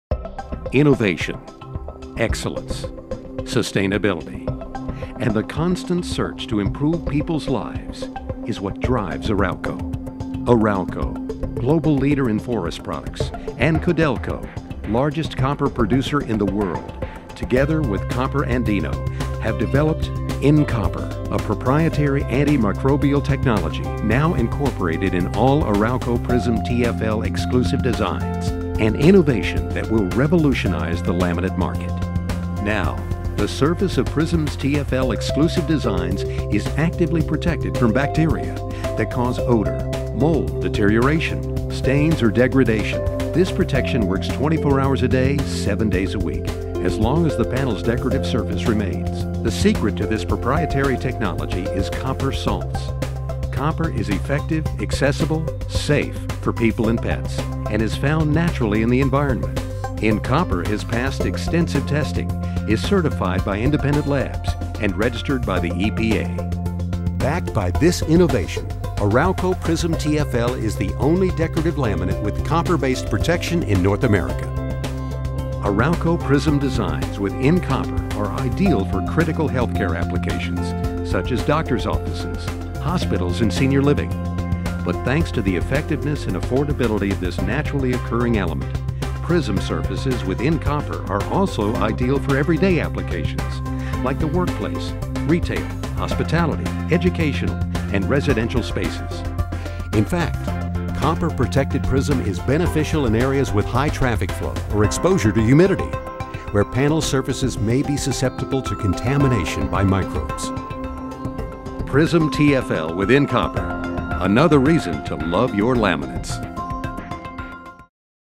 ARAUCO PRISM TFL with InCopper Antimicrobial technology
Arauco introduces InCopper® antimicrobial technology.  This proprietary technology incorporated in all Prism TFL Exclusive designs, protects the surface of the panel, actively  inhibiting the growth of microorganisms that cause deterioration, stains or degradation.
Prism TFL, the only laminate with antimicrobial copper protection in North America.